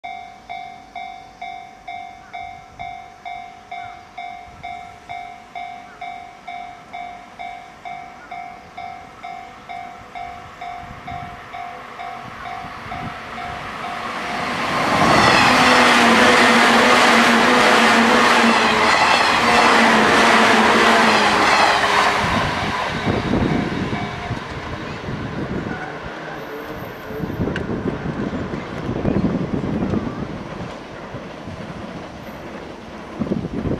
JR東海道本線185系A1編成特急「踊り子」東京行き 川崎駅付近の踏切通過
JR東海道本線川崎駅付近の踏切を通過する185系A1編成の特急「踊り子」東京行きです。撮影日は2018年8月4日です。

185系は長年「踊り子」で使用されてきましたが、いよいよ置き換えの時期がやってきました。西村京太郎サスペンスの舞台にもなったこの形式が引退すると、20年くらい後になって本の表紙を見たら、「この車両何？」とか言われるようになっちゃうんですかねぇ・・・

ちなみに185系は、A編成が「踊り子」の基本編成で使用されるため、撮影はしやすいほうです。ついで付属編成のC編成や定期で運用があるOM編成が撮影しやすいです。B編成は波動用であり、「ムーンライトながら」含めあっちこっちに神出鬼没なので撮影はちょっと難しいです。ちなみに廃車はB編成が早そうとのこと。

This is a Limited-Express train "ODORIKO" for Tokyo.I recorded this movie at the crossing near Kawasaki Station in Kanagawa Prefecture.